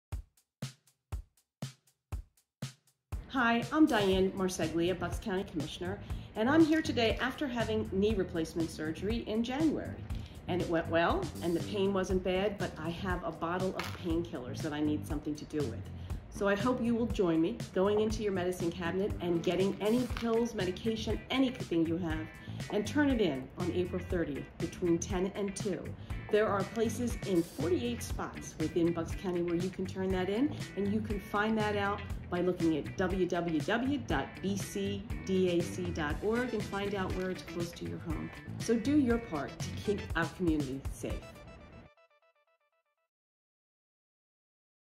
National Prescription Drug Take Back Day - Saturday, April 30, 2022
In this video, Bucks County Commissioner Diane Marseglia encourages Bucks County residents to safely dispose of their unneeded, unused, expired medications on Saturday, April 30. Details